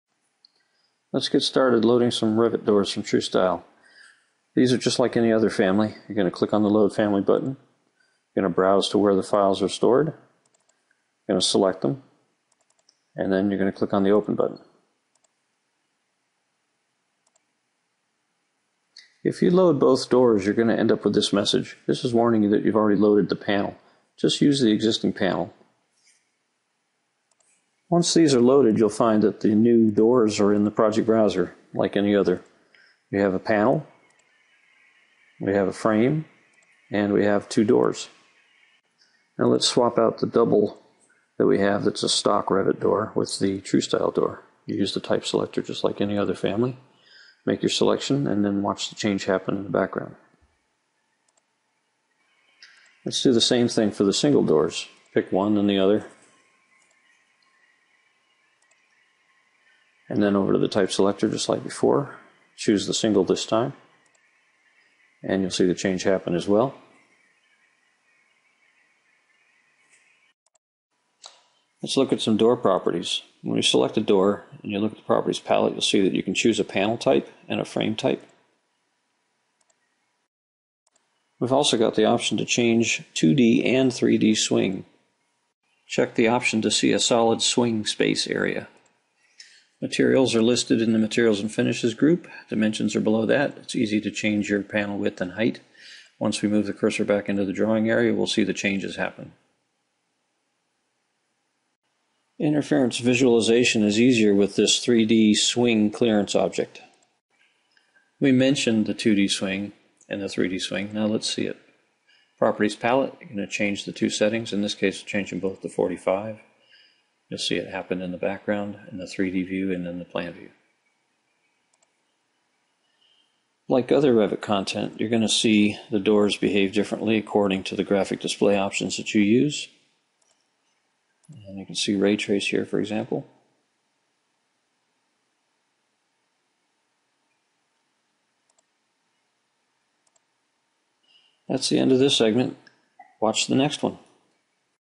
TruStile Revit BIM Door Models
The TruStile Revit families provide additional features that are not available in stock door families. TruStile Revit families are parametric models that let users modify door height, width and thickness, 14 wood species or MDF, 11 sticking and 6 panel options, substitution of a panel for clear or frosted glass plus specifying the exact height, width and location of all the stiles and rails on a door.